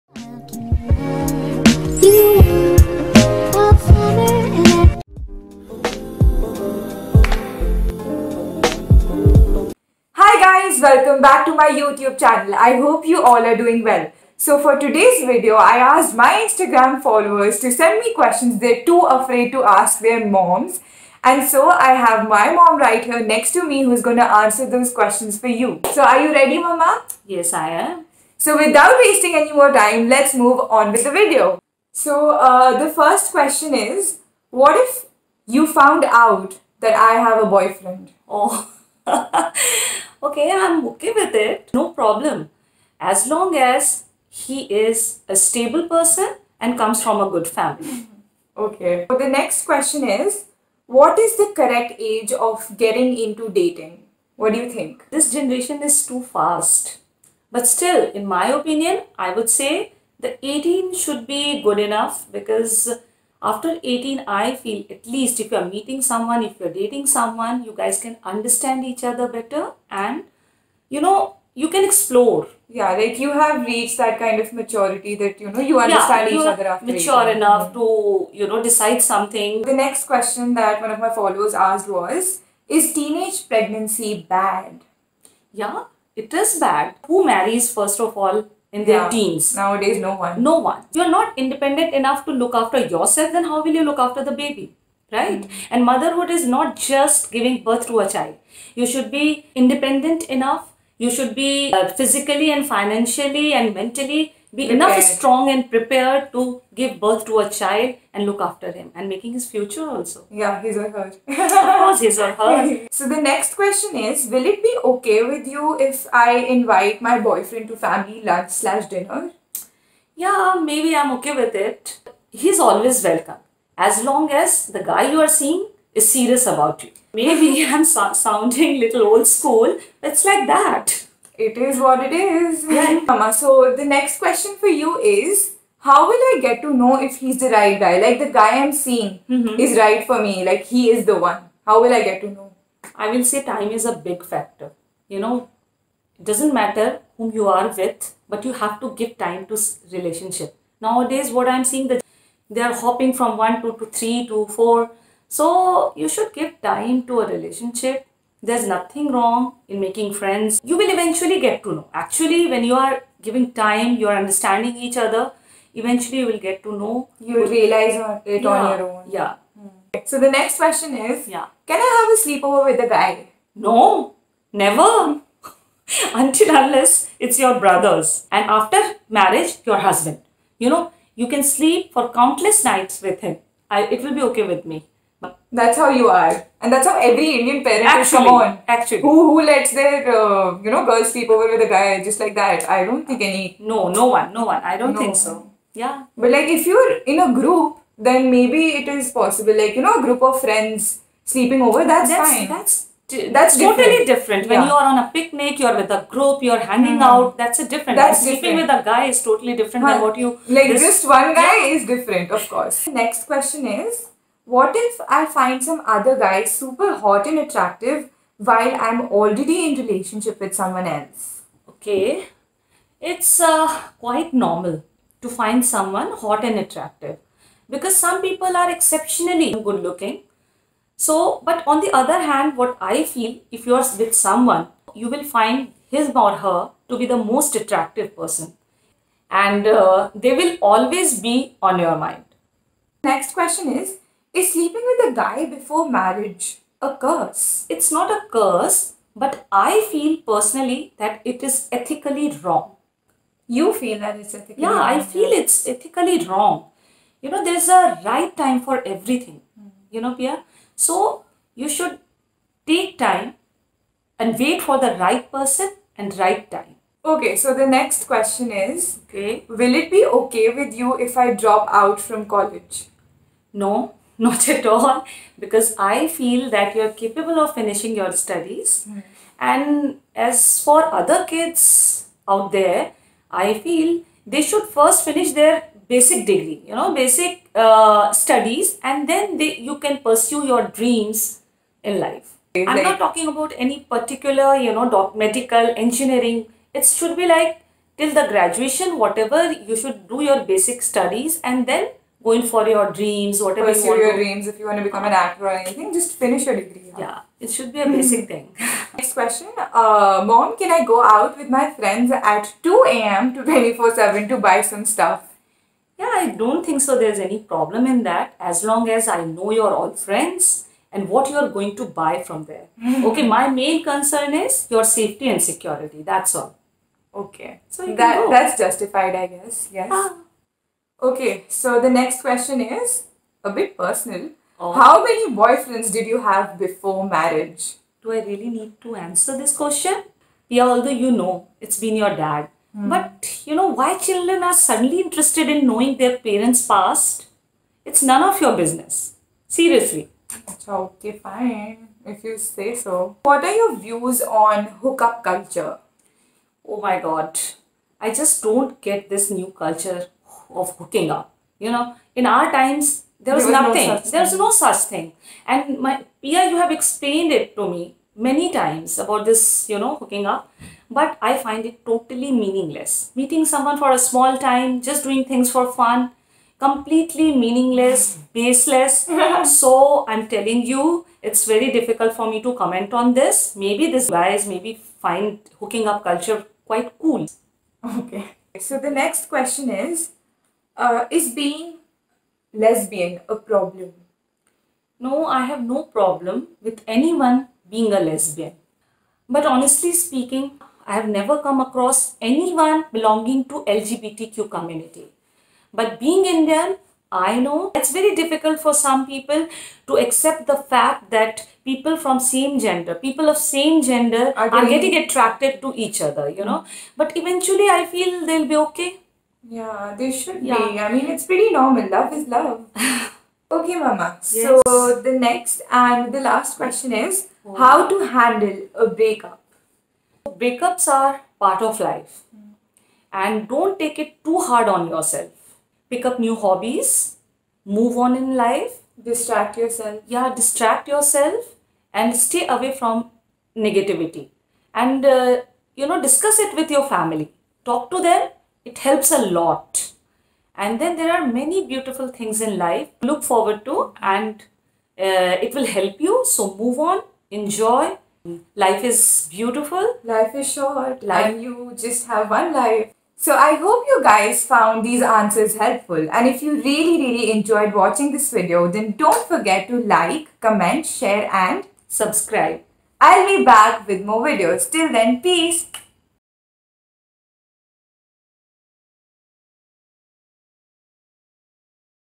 ASKING MY MOM QUESTIONS YOU'RE TOO AFRAID TO ASK YOURS!!
A few days back i had asked my friends and followers on Instagram to send me questions they were too afraid to ask their own mom, so that my mom can answer those questions and help them out hehe